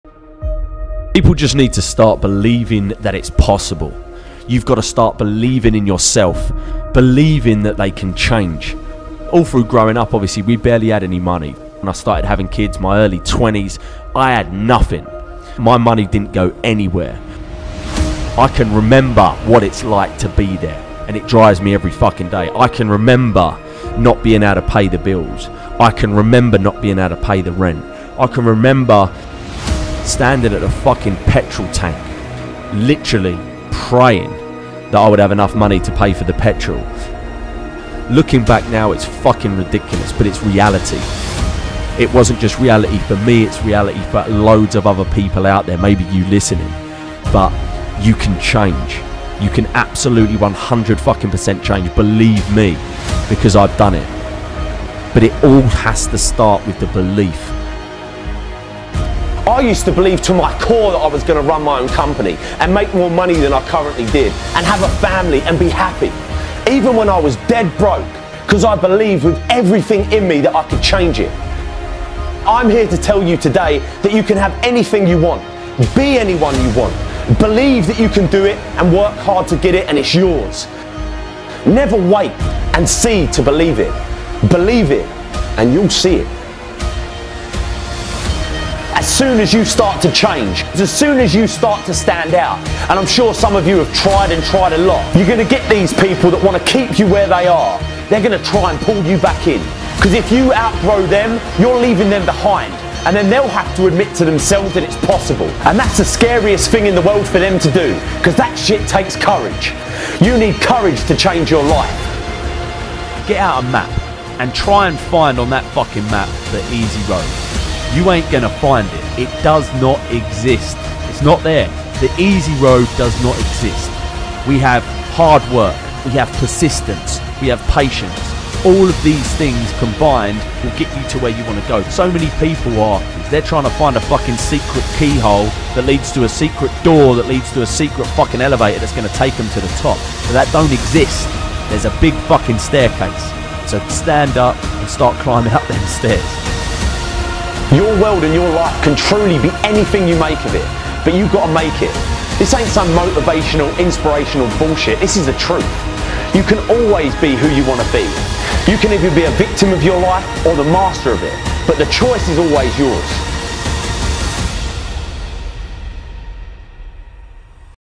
Believing In Yourself - Best Motivational Speech 2022
Believing in yourself

Start believing in yourself.  You can change, you can change your life.  Just start to believe in yourself.  It is possible, it is possible to be you want to be.  Believing in yourself is what you will need to do.  You can make your dream a reality, you just need to start by believing.

Speakers: Mat Wilson

Edited By: @billyBonesProductions